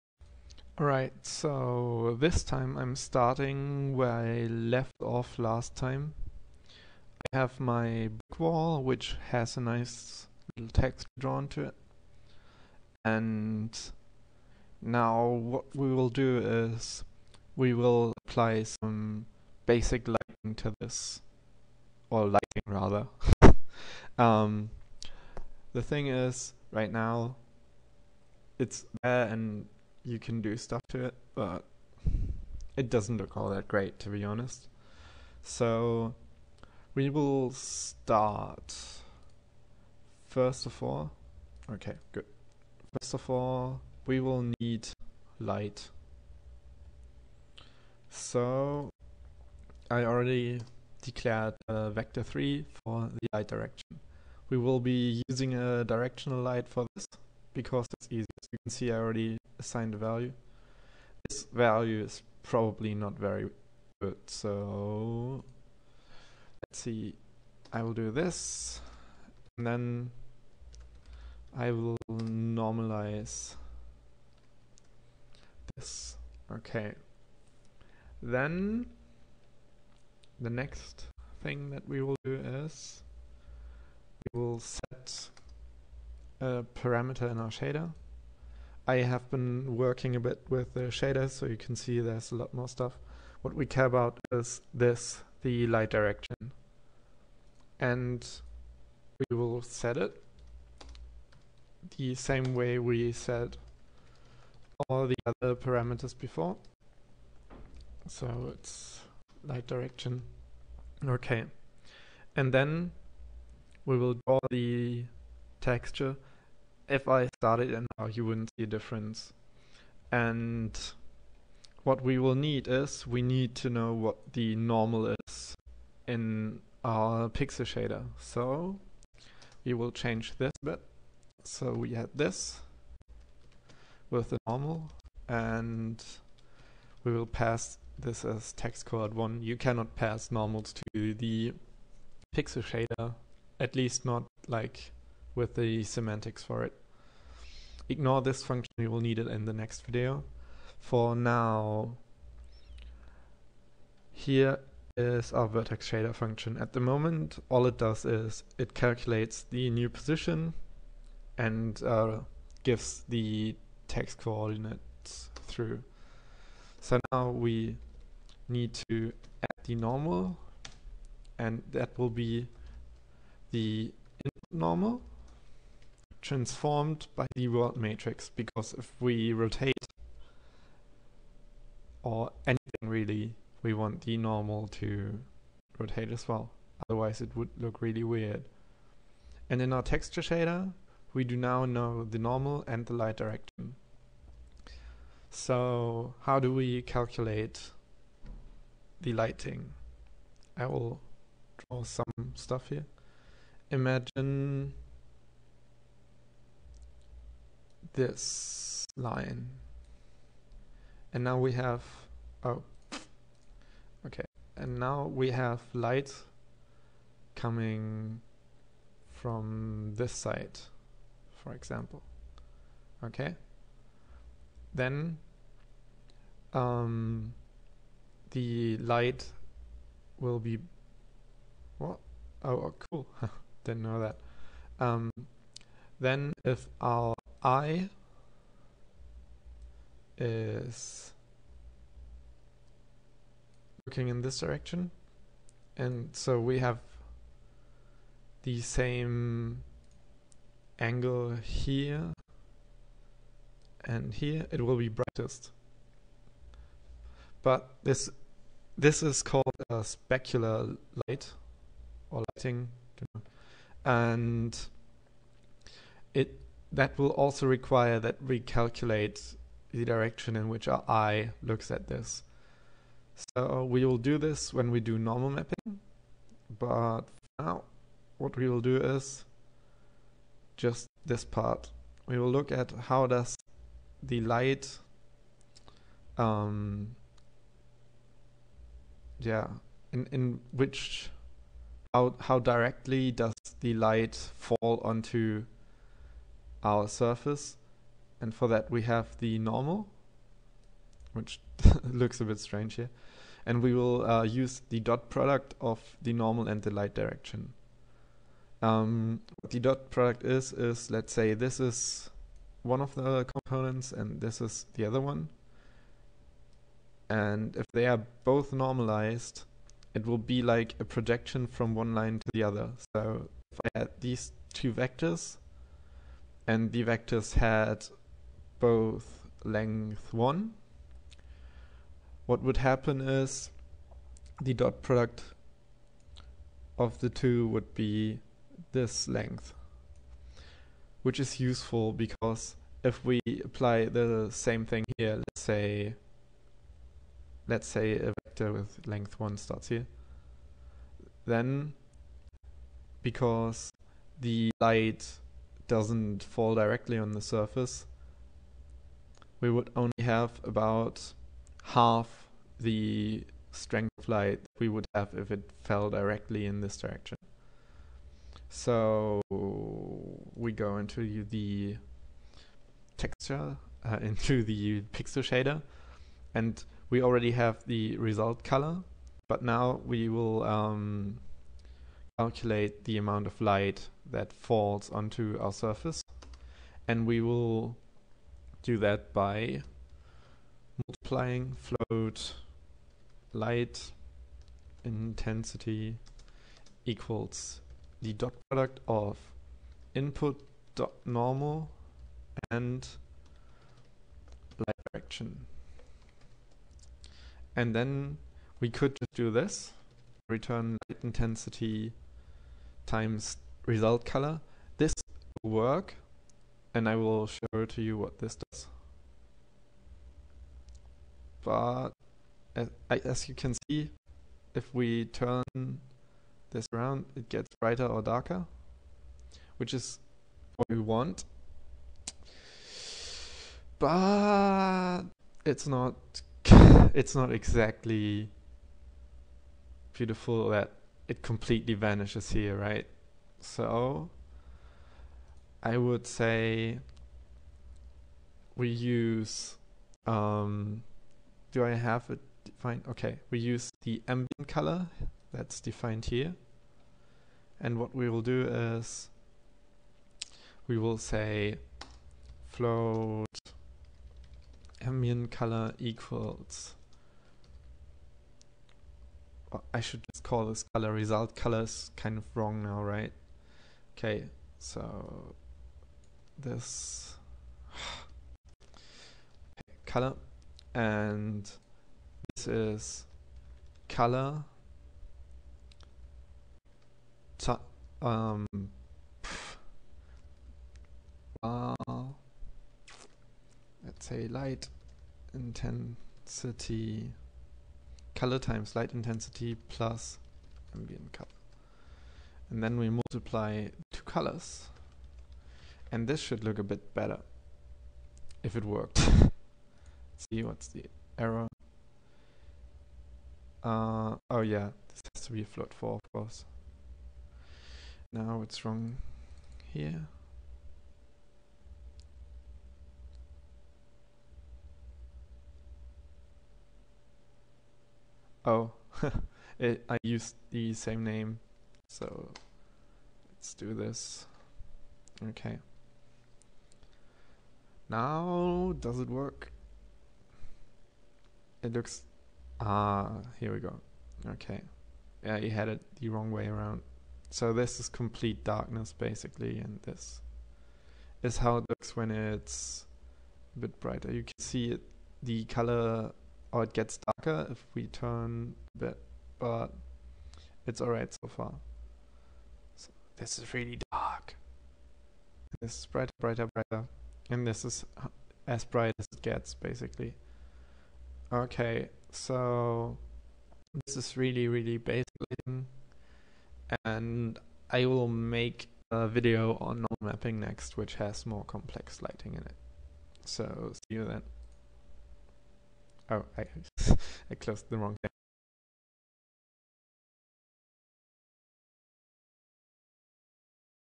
How To: Basic Lighting in XNA 4.0 with Shaders
Here is a very small video that is ment to explain how to use very basic lighting with XNA 4.0 and shaders.
Hopefully the audio isn't off as much this time.
Also I need to make a correction.

Of course you multiply the Normal with the negative lightDirection because otherwise the light would have to shine from behind to generate a positive light value.